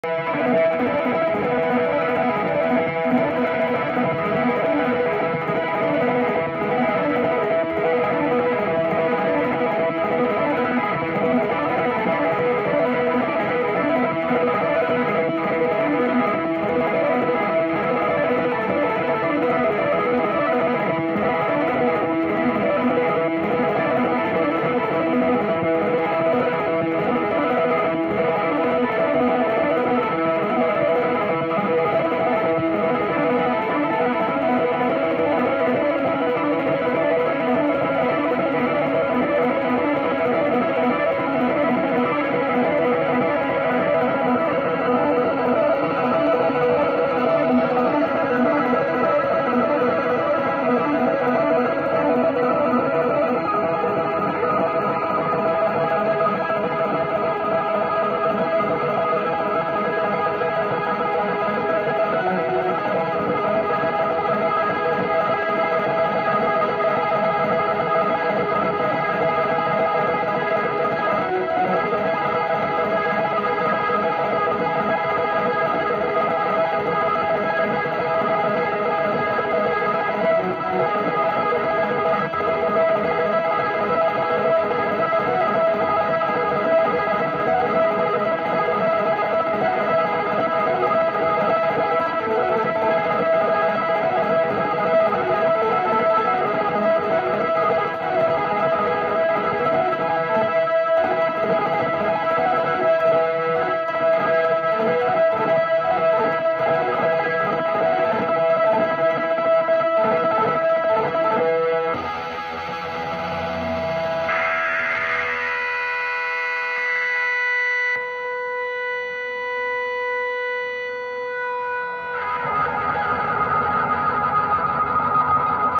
Bill Nace on taishōgoto
February 3, 2023 at Brick House in Turners Falls, MA | Video by Jake Meginsky

Bill Nace performs on taishōgoto for the first time on the West Coast at Zebulon Café Concert in Los Angeles Sunday on February 19th, presented by UPEND.